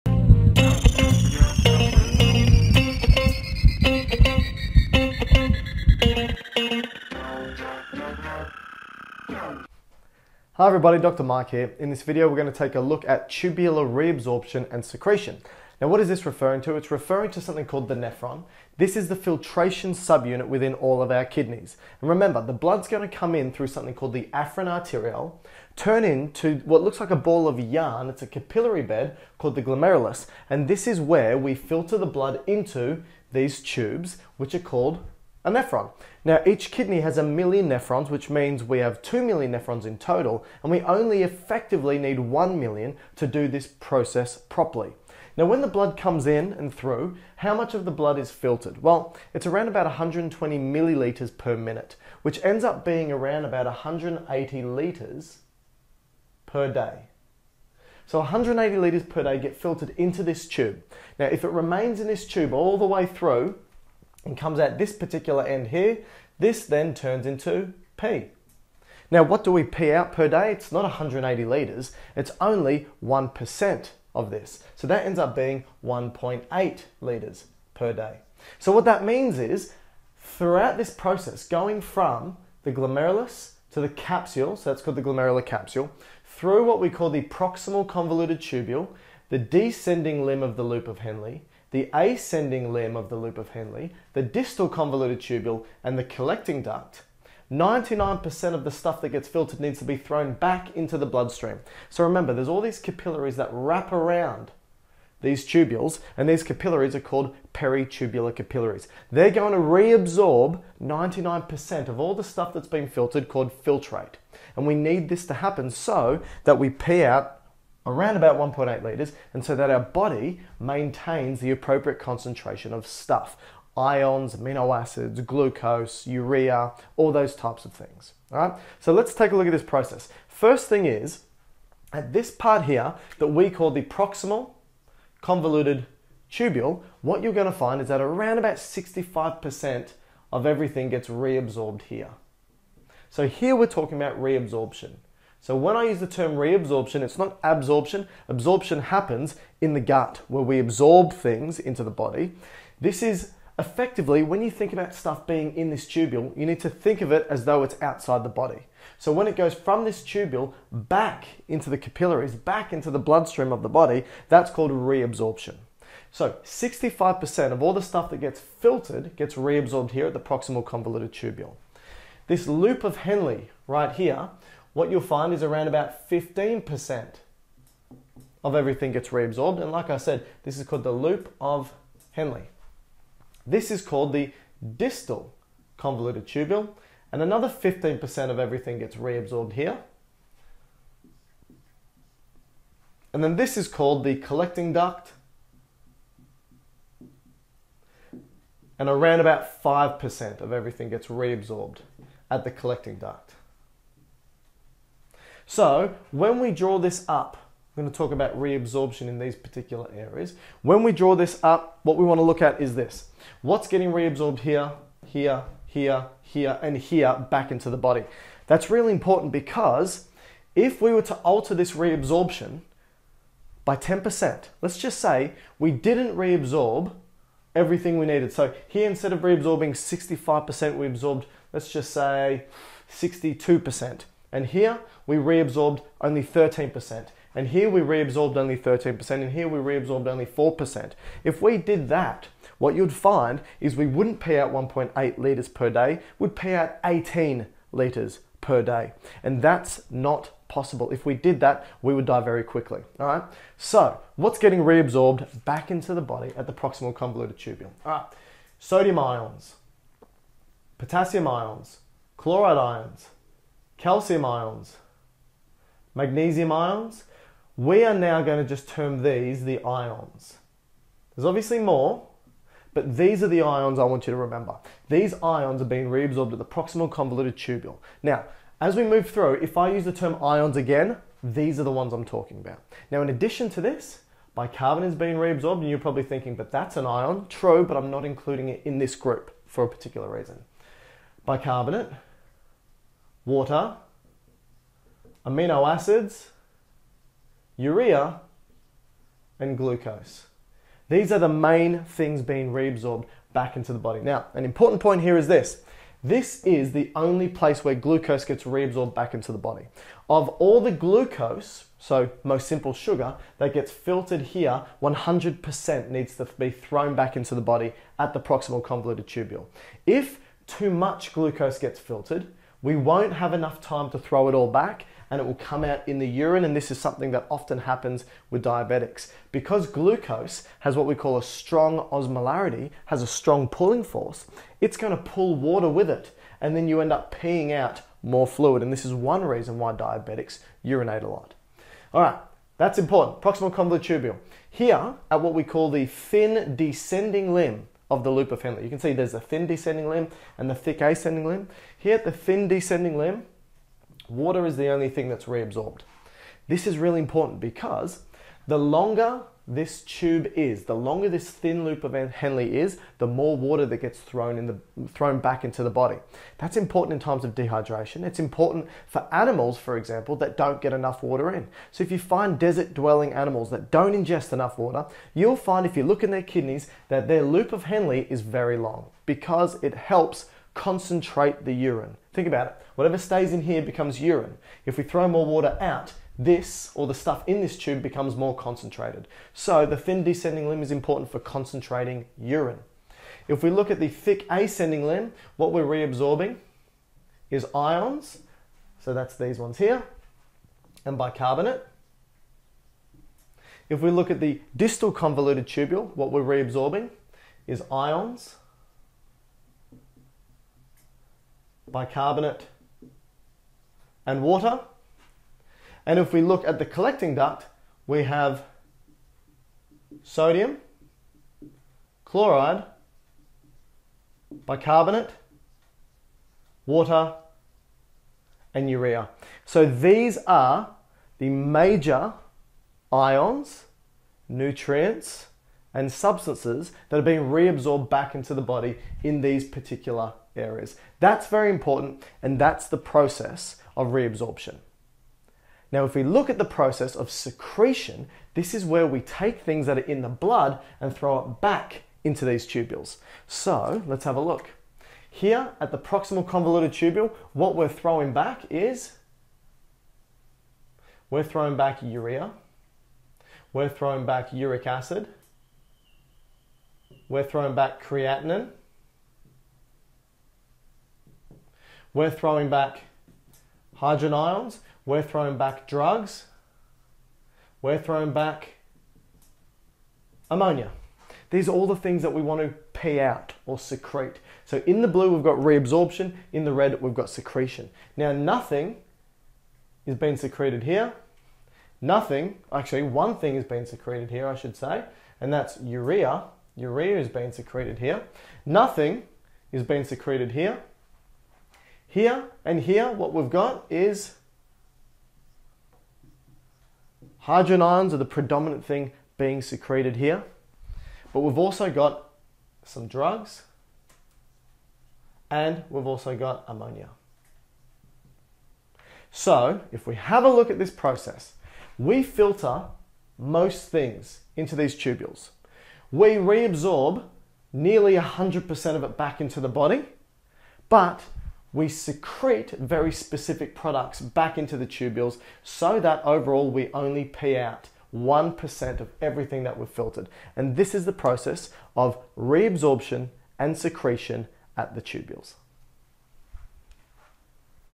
Tubular Reabsorption & Secretion | Renal System
In this video, Dr Mike explains what components of filtrate get "reabsorbed" back into the body and what components of blood get "secreted" into the tubules of the nephron. He also shows you where along the nephron each substance gets either reabsorbed or secreted.